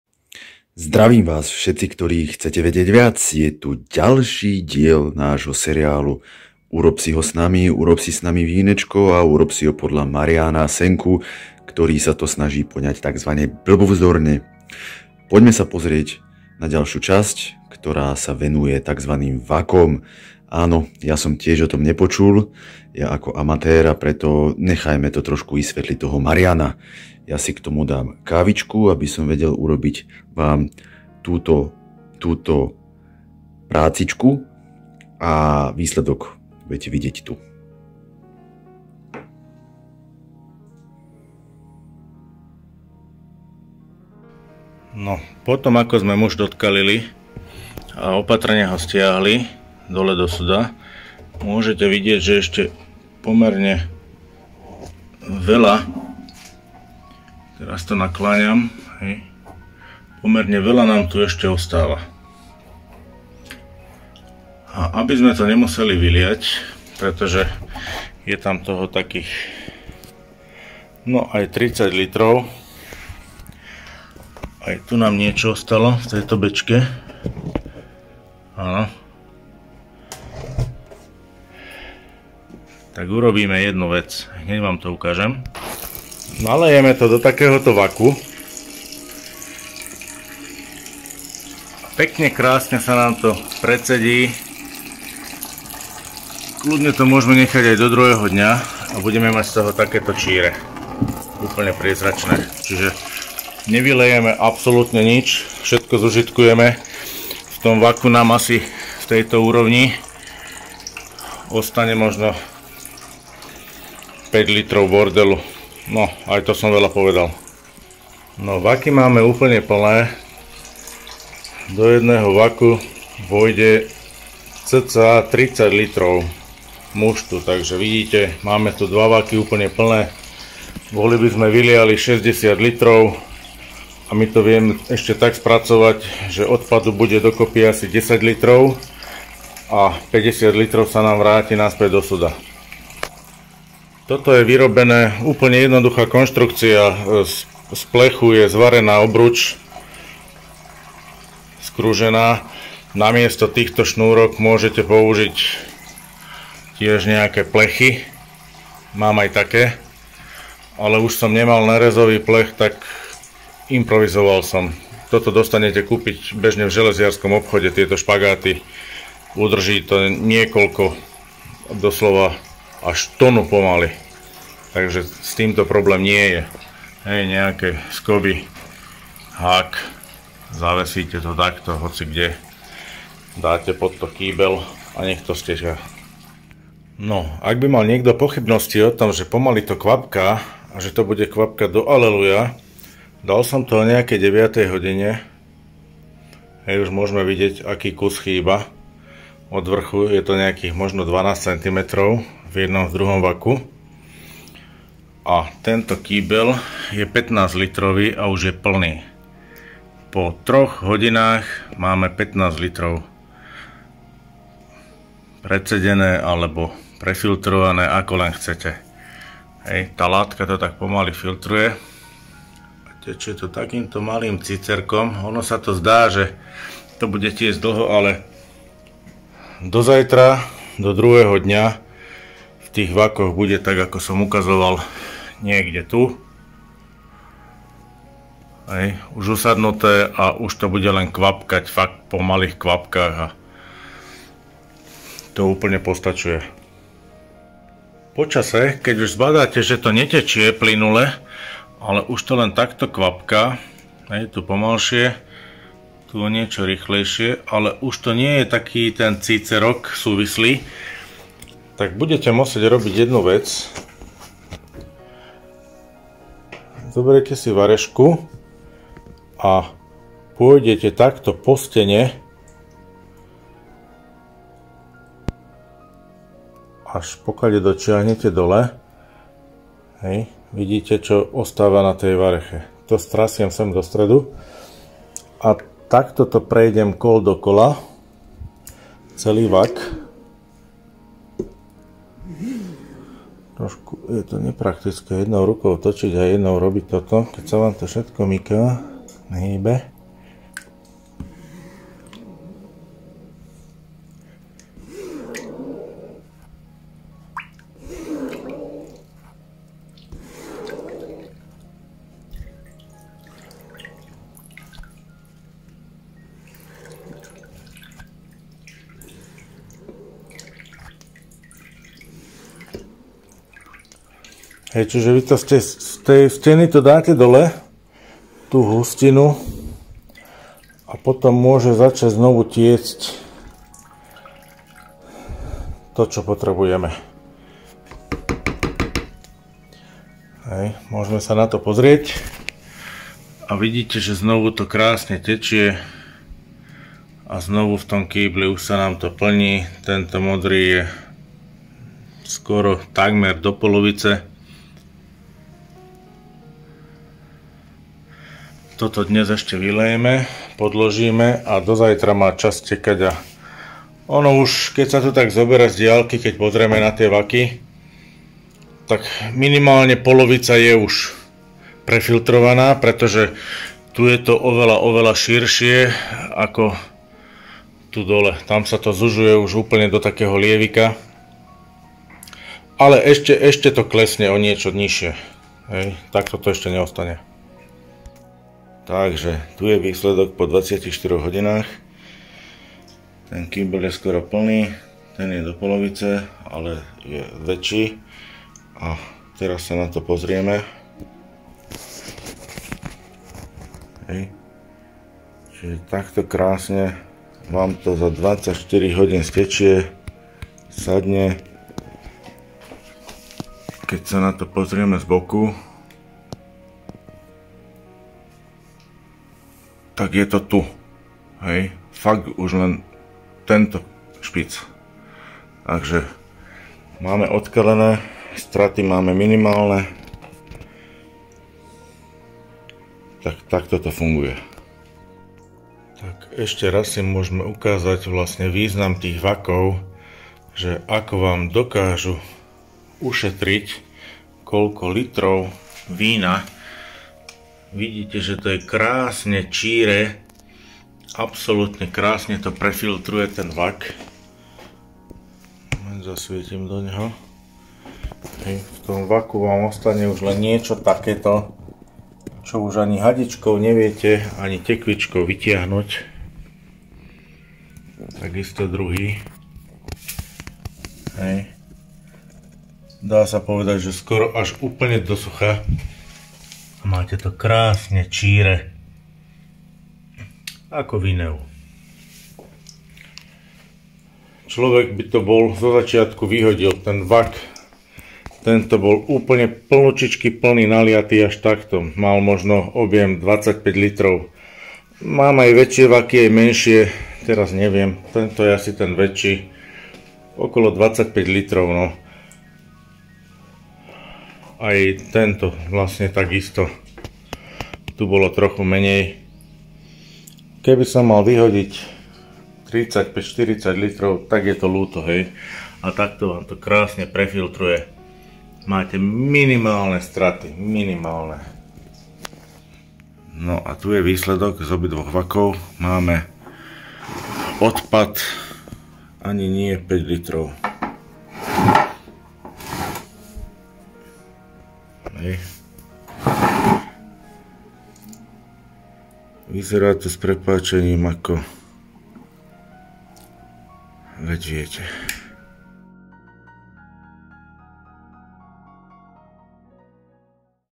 VÍNO a... UROB SI HO s nami - VAKY
Ďakujem Mariánovi Senkovi, že sa na to dal a mal snahu a chuť nás tzv. "blbuvzdorne" sprevádzať výrobou vína. Aj nás, tých najväčších amatérov v tejto oblasti (Teda ja určite :D ).
Vínku teda zdar a šup ho klik na video :).
Podporiť Mariána svojím darom môžete prostredníctvom tohto účtu:
SK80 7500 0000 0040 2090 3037